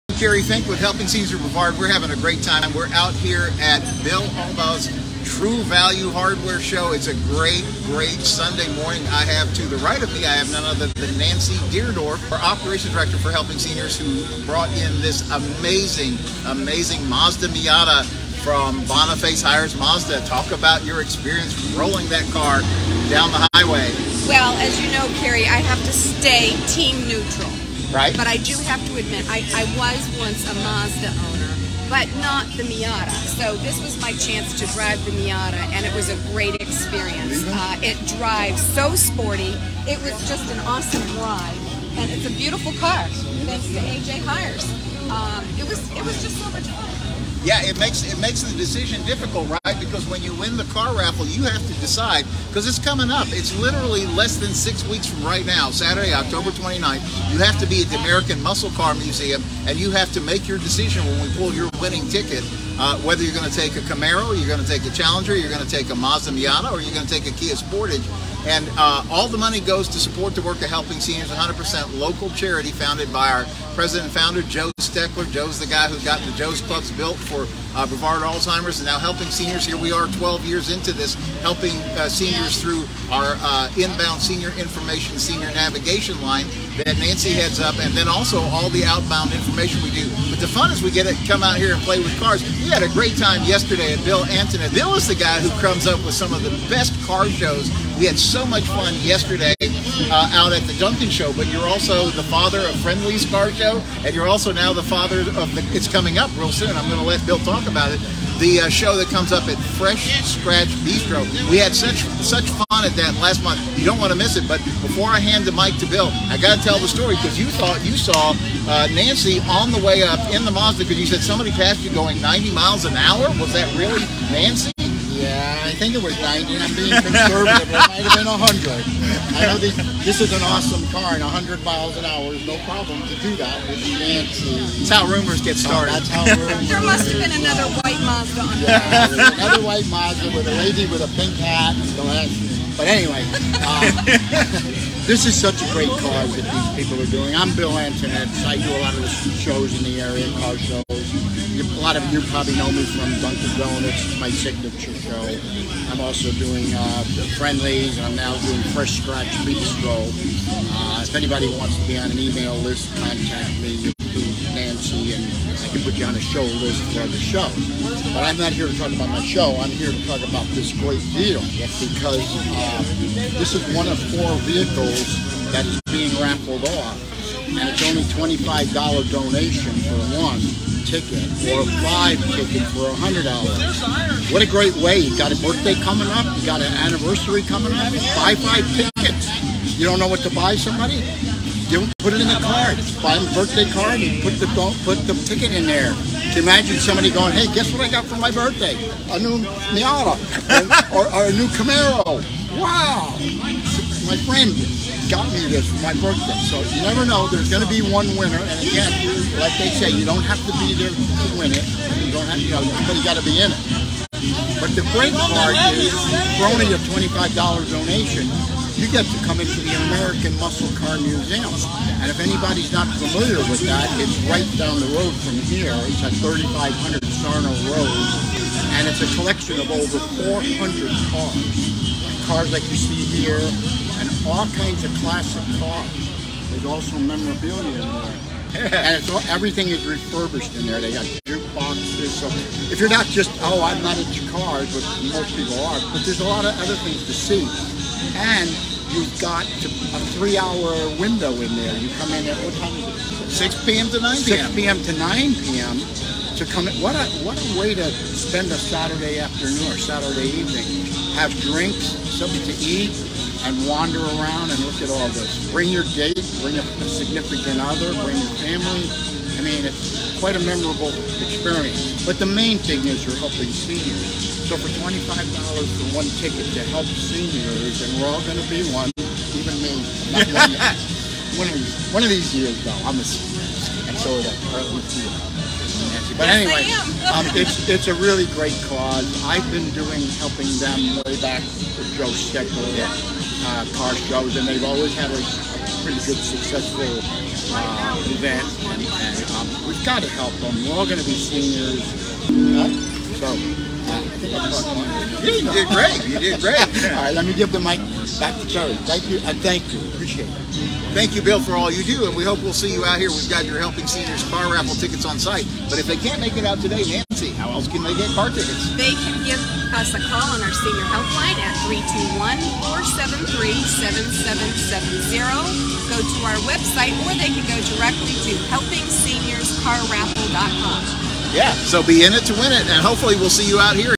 Bill Antonetz at True Value Car Show | Helping Seniors Car Raffle
Kerry Fink (Executive Director) and Nancy Deardorff (Operations Director) on Location at Bill Albaugh's True Value / Boston Market Car Show and visiting with Bill Antonetz, who organizes the monthly Dunkin' Donuts Car Shows, Friendly's Car Shows and the Fresh Scratch Bistro Car Shows.

All of the visit is in support of the Helping Seniors Car Raffle for Charity.  Each ticket (1 for $25 or 5 for $100 donation) is also your "Admit One" to the American Muscle Car Museum on the night of the Grand Drawing, Saturday, October 29th 2022.

Thanks also to our friends from CarePlus Health Plans for supporting the work of Helping Seniors!

Get your Helping Seniors Car Raffle Tickets and Be in it Win it!

Info on Helping Seniors Car Raffle:
HelpingSeniorsCarRaffle.com